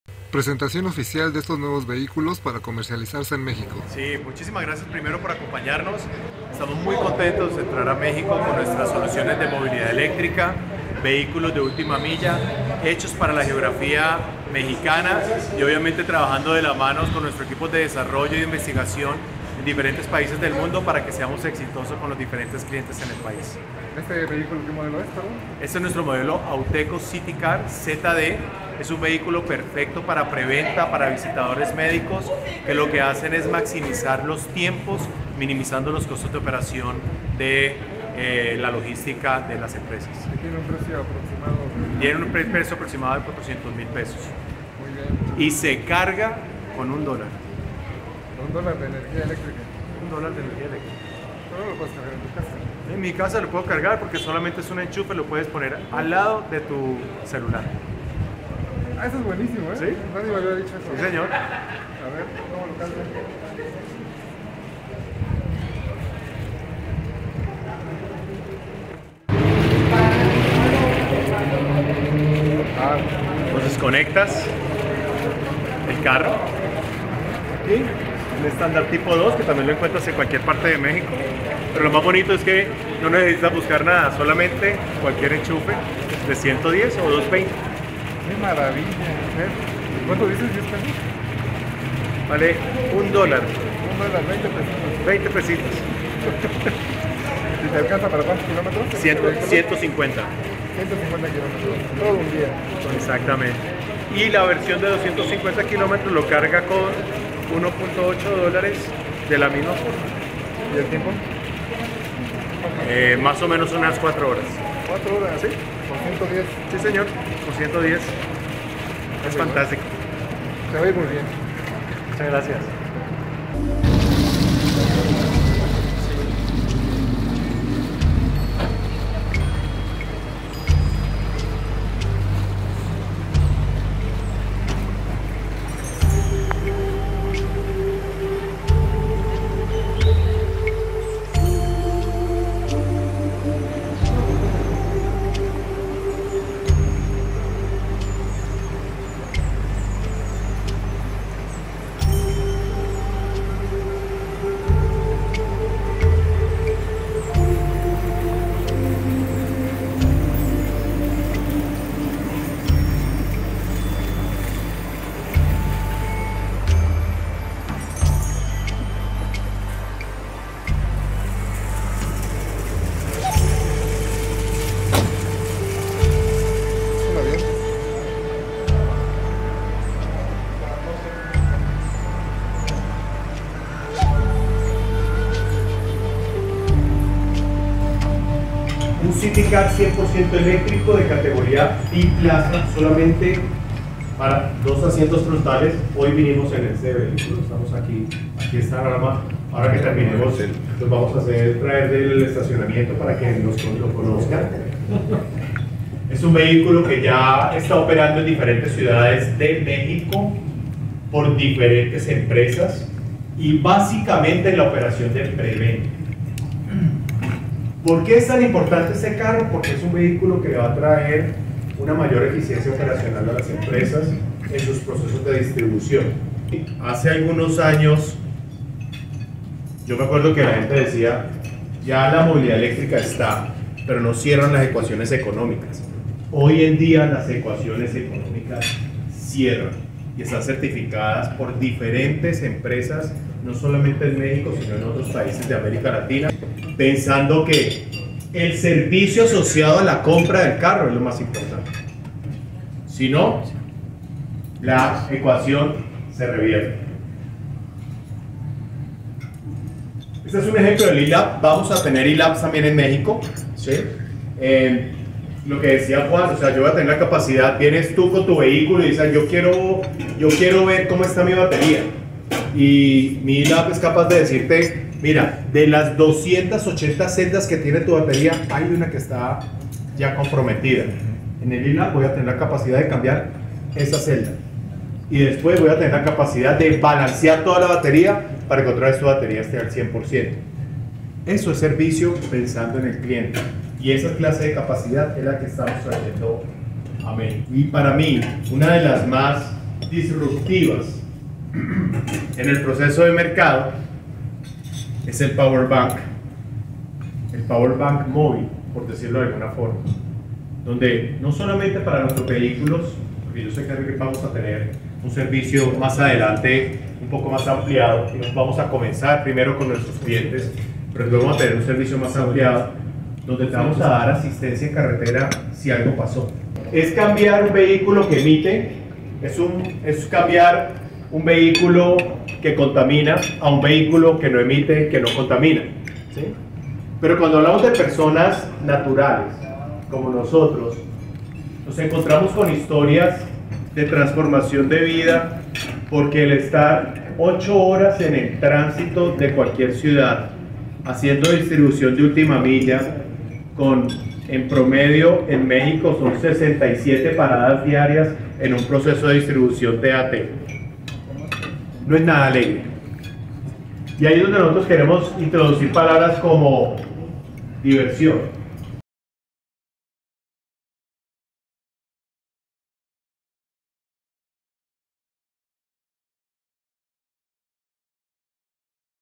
Auteco México presenta e- City Car ZD, que con $20 pesos de costo, se recarga su batería/ 150Km
El auto más importante de la empresa colombiana en México, es el Modelo Auteco City Car ZD biplaza, enfocado para preventa y última milla, con un precio de alrededor de 400 mil pesos mexicanos.

Una de las características importantes de este vehículo, es que puede recargarse en casa, o en cualquier lugar que cuente con tan sólo un enchufe normal de 110V; y en 40 minutos, se obtiene una autonomía de 150 Kilómetros con un costo promedio de $20.00 pesos mexicanos (1 Dolar).

La versión de 250 Km, se calcula que tiene un costo actual de $38.00 pesos mexicano por carga completa. (1.8 USD)

Andrés Cortés Parra, country manager de Auteco en México mencionó en conferencia, que sin duda, el servicio asociado a la compra del vehículo, es lo más importante.

Autotécnica Colombiana anunció una inversión de más de 100 millones de dólares en un plazo no definido para consolidarse en el mercado mexicano de vehículos eléctricos.

La empresa proyecta contar entre otras cosas, con Laboratorios Eléctricos y servicio ampliado para clientes en carretera.

"Diversión", es unos de los conceptos con los que la comercializadora impulsará al eléctrico City Car ZD de Auteco.

9 de Noviembre 2023.